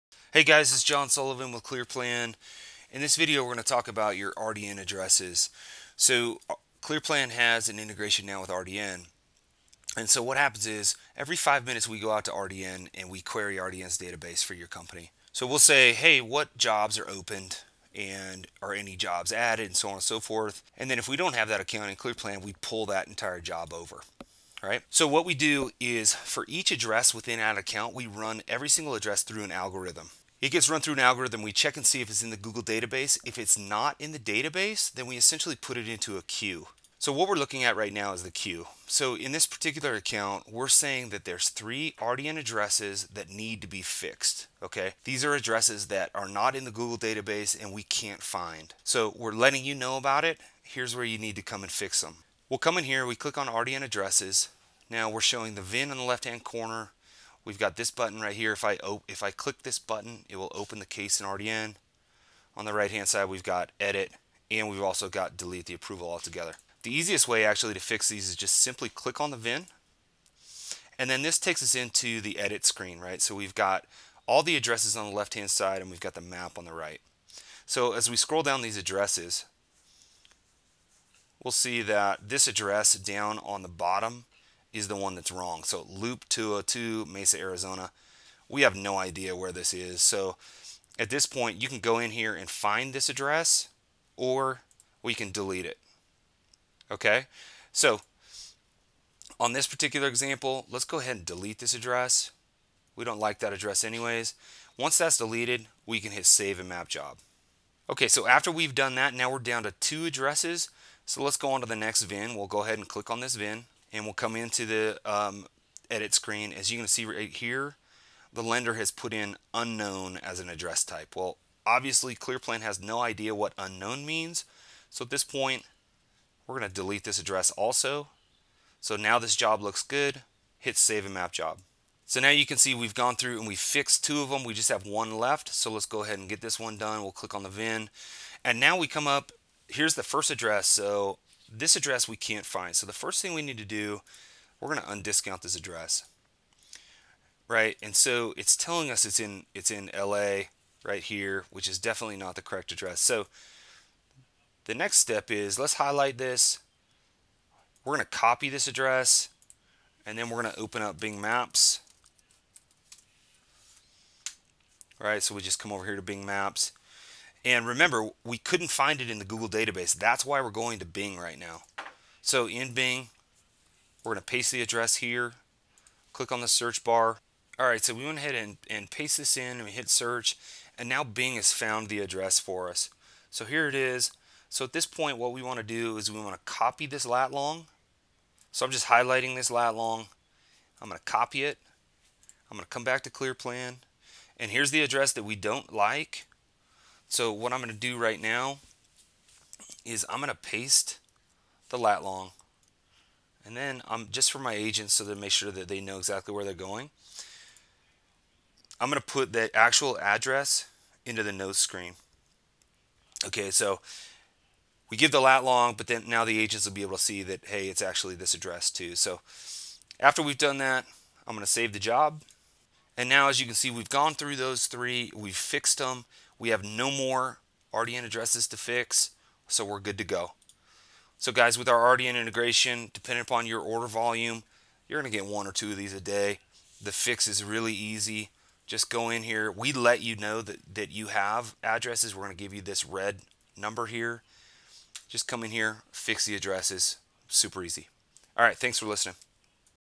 Validating RDN Addresses within Clearplan
In this video, you will learn how to validate and map RDN addresses in Clearplan.